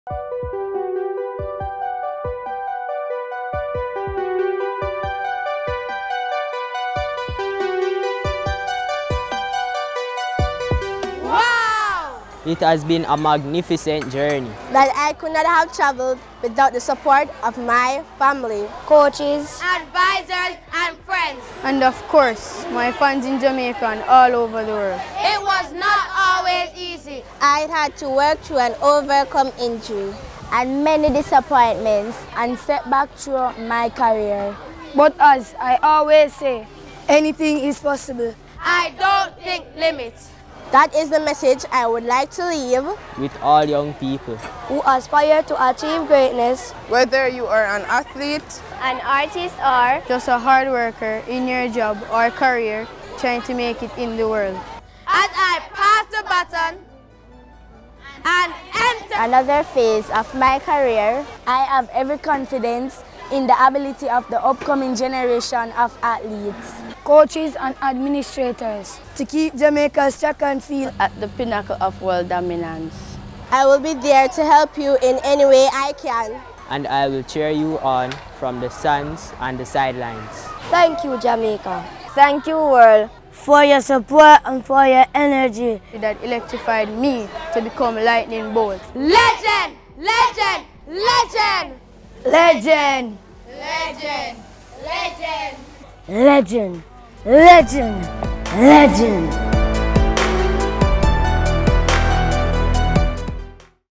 The Finish Line - Usain Bolt's Personal Farewell
As he approached the finish line of his career, Usain Bolt penned a personal farewell at the end of Usain Bolt: Legend. Hear it recited by track and field teams from Naggo Head Primary, Hydel Preparatory and Bolt's alma mater Waldensia Primary.